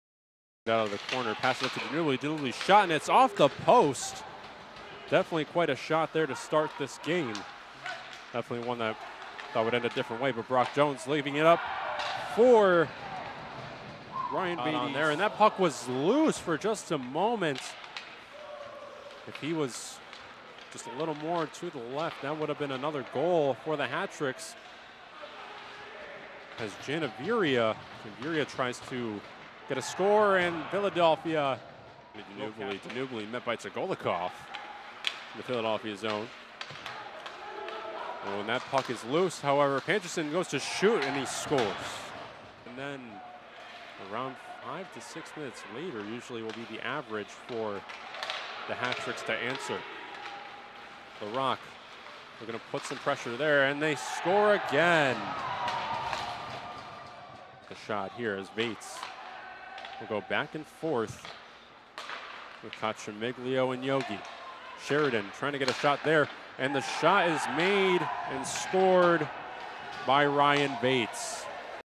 Rebels Game Highlights vs. Danbury Hat Tricks - February 17, 2024
Ryan Vaites scored a powerplay goal with 1:46 remaining in overtime to give the visiting Rebels a 5 – 4 win over Danbury to complete the weekend sweep. It was his second goal of the game. Charles Panchisin, Jared Coccimiglio and Otto Polakovs also scored in regulation.